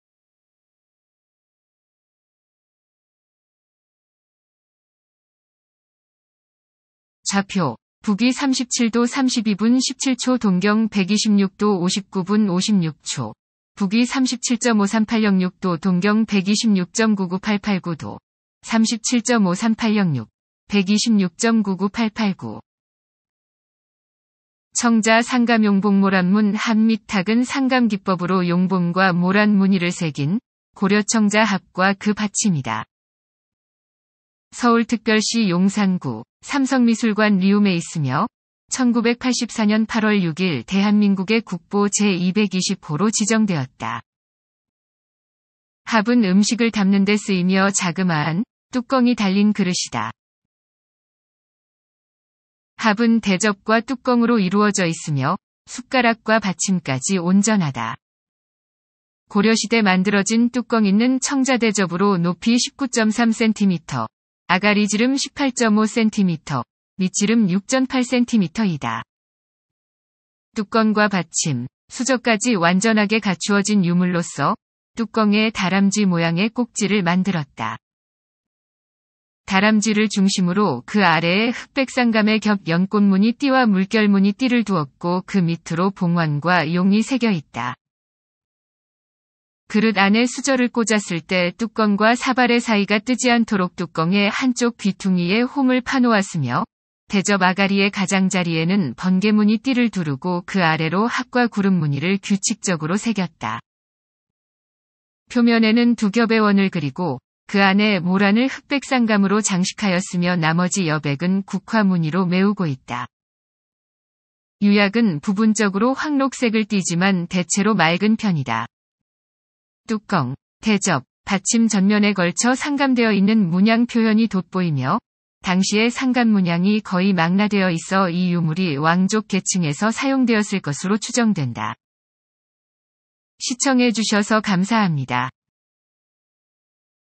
청자 상감용봉모란문 합 및 탁
좌표: 북위 37° 32′ 17″ 동경 126° 59′ 56″ / 북위 37.53806° 동경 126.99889°  / 37.53806; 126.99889
청자 상감용봉모란문 합 및 탁（靑磁 象嵌龍鳳牡丹文 盒 및 托）은 상감기법으로 용봉과 모란 무늬를 새긴, 고려청자 합과 그 받침이다.
서울특별시 용산구, 삼성미술관 리움에 있으며, 1984년 8월 6일 대한민국의 국보 제220호로 지정되었다.
합은 음식을 담는 데 쓰이며 자그마한, 뚜껑이 달린 그릇이다.
합은 대접과 뚜껑으로 이루어져 있으며, 숟가락과 받침까지 온전하다.
고려시대 만들어진 뚜껑있는 청자대접으로 높이 19.3cm, 아가리 지름 18.5cm, 밑 지름 6.8cm이다.
뚜껑과 받침, 수저까지 완전하게 갖추어진 유물로서, 뚜껑에 다람쥐 모양의 꼭지를 만들었다.
다람쥐를 중심으로 그 아래에 흑백상감의 겹 연꽃무늬 띠와 물결무늬 띠를 두었고 그 밑으로 봉황과 용이 새겨있다.
그릇 안에 수저를 꽂았을 때 뚜껑과 사발의 사이가 뜨지않도록 뚜껑의 한쪽 귀퉁이에 홈을 파 놓았으며, 대접 아가리의 가장자리에는 번개무늬 띠를 두르고 그 아래로 학과 구름무늬를 규칙적으로 새겼다.
표면에는 두 겹의 원을 그리고, 그 안에 모란을 흑백상감으로 장식하였으며 나머지 여백은 국화 무늬로 메우고 있다.
유약은 부분적으로 황록색을 띠지만 대체로 맑은 편이다.
뚜껑, 대접, 받침 전면에 걸쳐 상감되어 있는 문양 표현이 돋보이며, 당시의 상감문양이 거의 망라되어 있어 이 유물이 왕족 계층에서 사용되었을 것으로 추정된다.